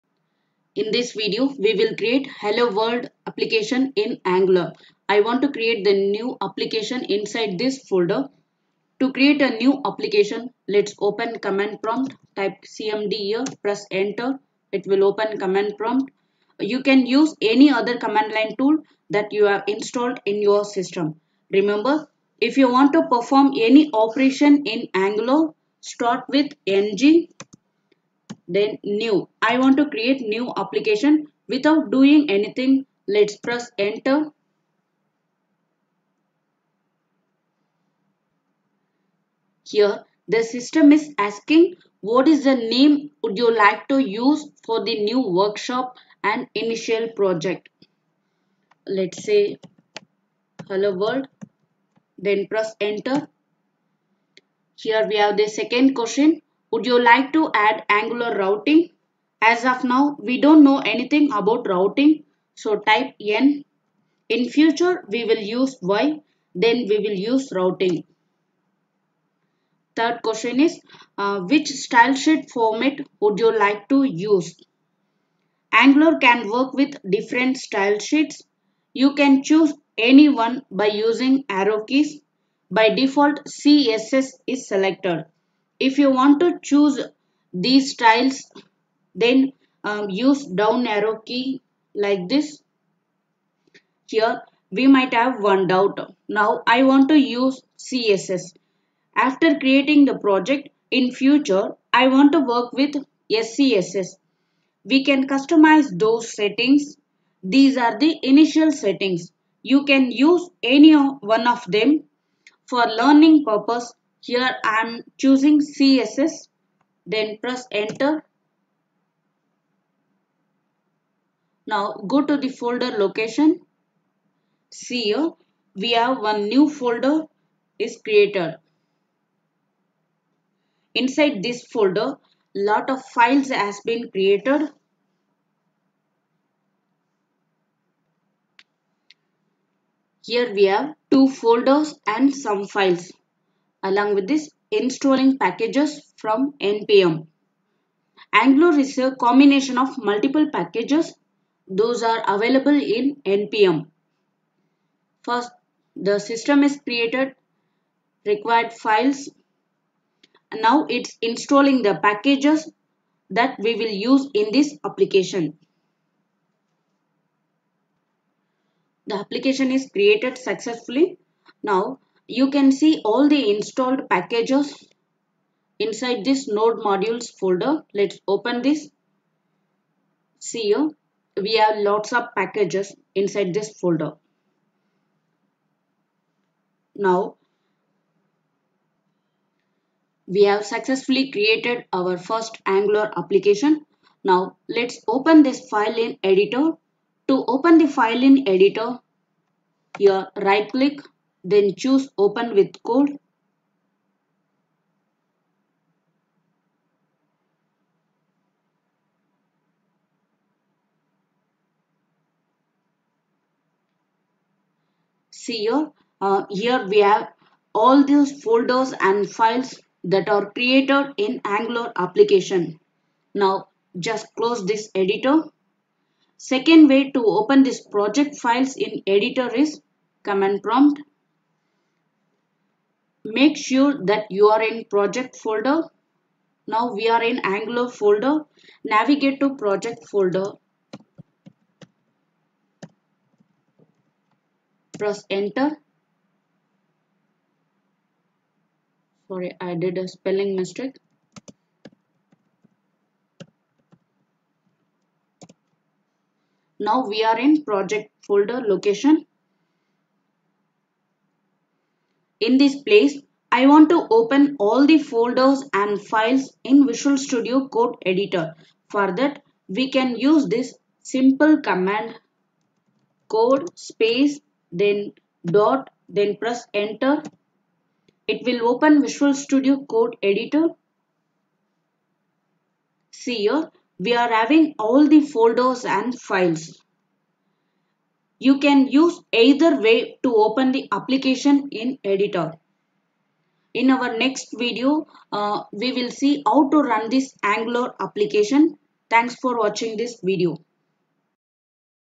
#2: Hello World App in Angular 14 | First Application in Angular 14
This is step by step tutorial. In this video we will learn how to create our first application in Angular 14.